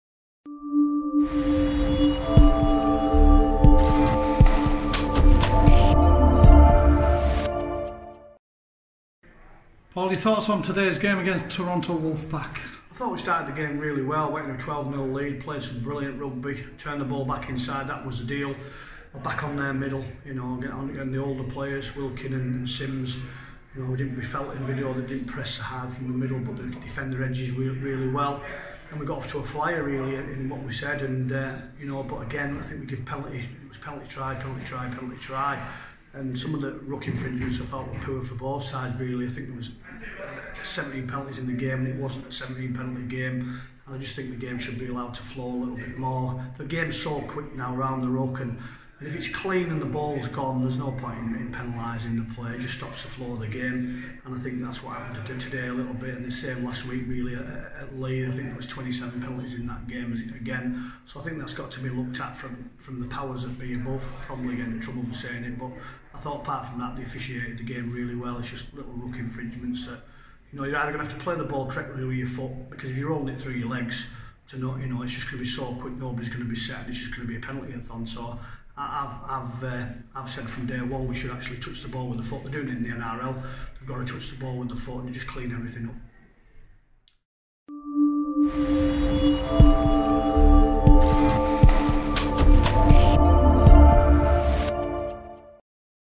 Interview: Paul Crarey Post-match Toronto Wolfpack
Paul Crarey gives his post match thoughts after Good Friday's game with Toronto Wolfpack